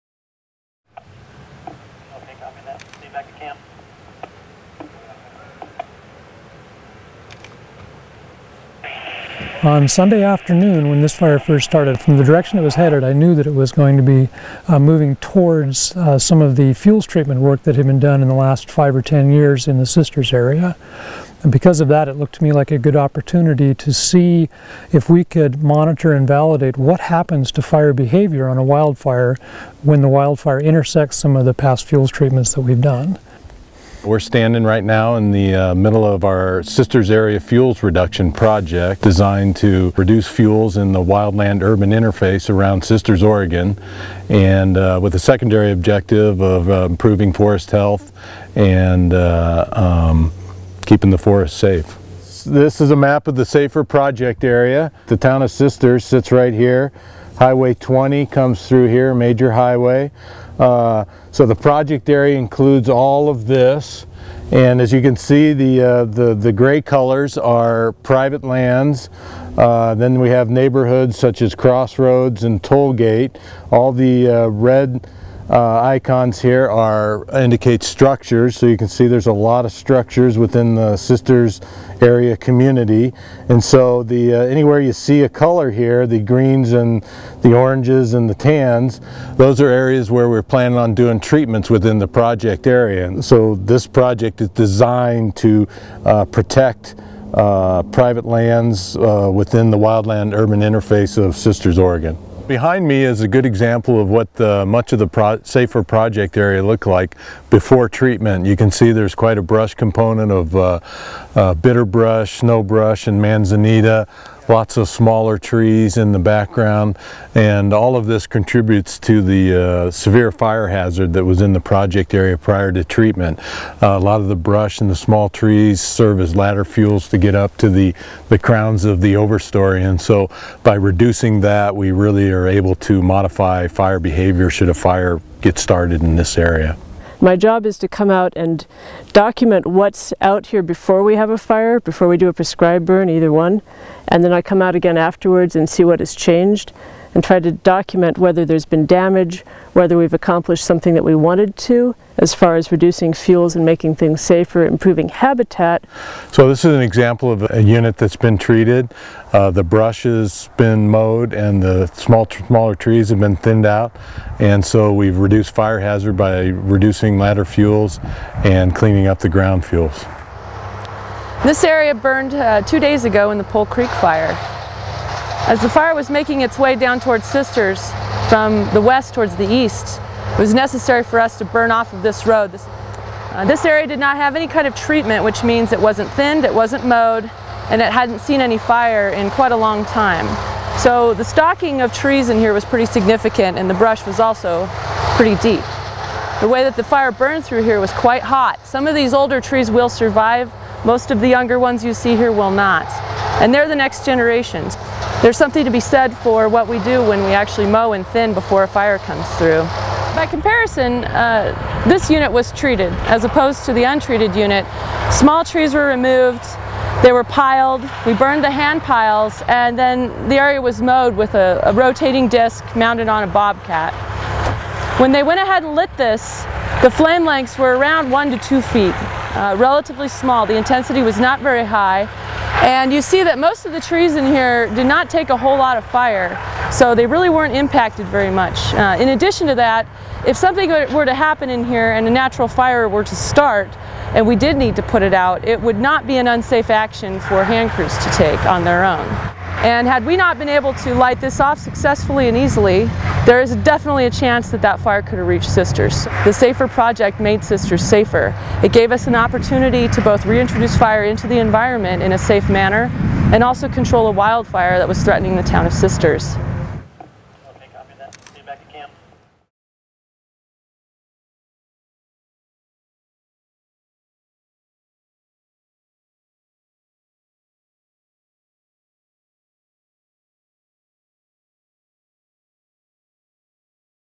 Make Wildland Urban Interface Communities Safer with Fuels Reduction
Video explaining fuel treatments near Sisters, Oregon and their role in controlling the Pole Creek fire and other fires in the Wildland Urban Interface.